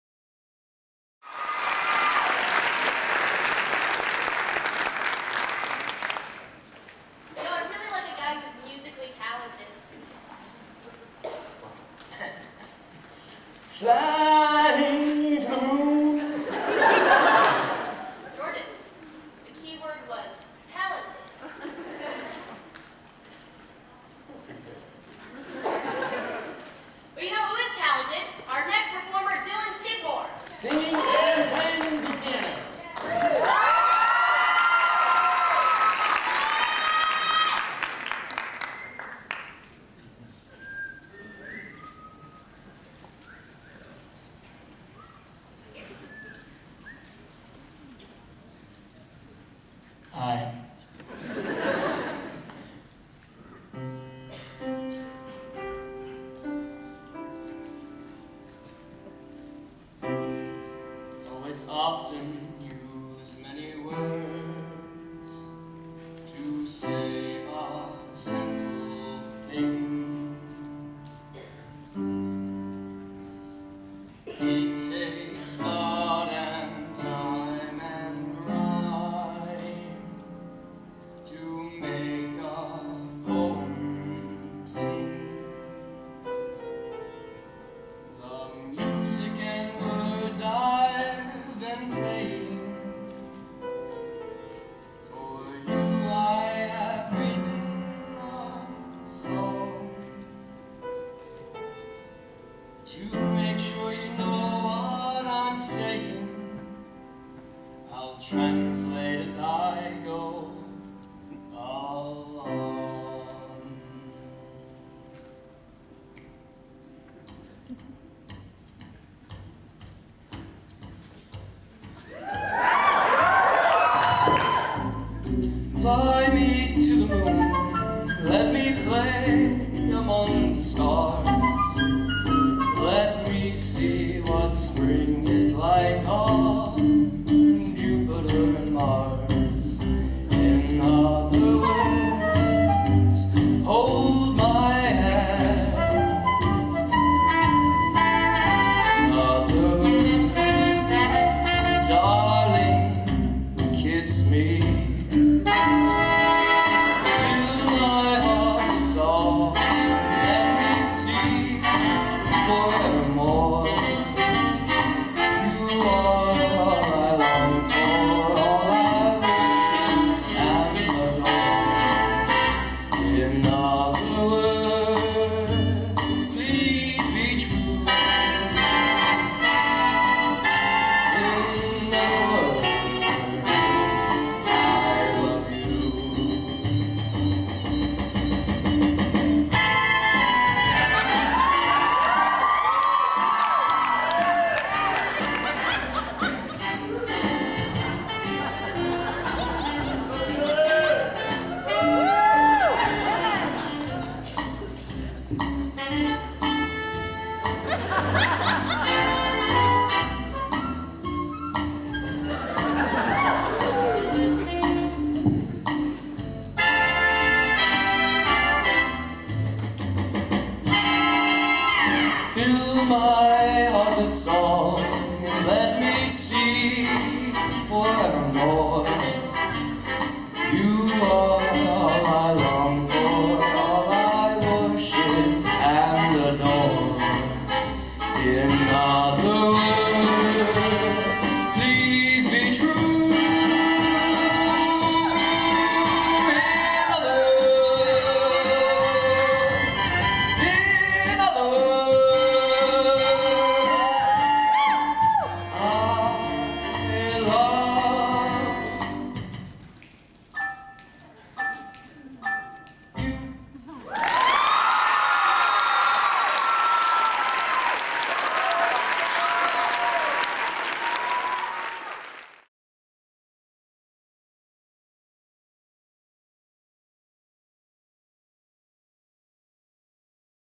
Mr. Mustang: A Little Diddy I Call A Song
Dylan Tidmore's talent is a performance of a song accompanied by some piano playing.

*Grapevine High School hosted the all-male beauty pageant, Mr. Mustang on February 25th
Contenders competed for three different categories:
Mr. Swimsuit
Mr. Congeniality
and of course, Mr. Mustang.